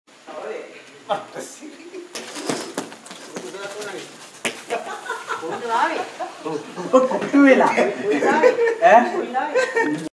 Kabaragoya chased from the house
The Kabaragoya was really stubborn and didn’t want to get out of the house. Selvam, one of our workers, eventually managed to chase him out. He is experienced with them, and told us that he once had his foot torn apart by one. We had 3 guys working there that day, and they were all having a good laugh.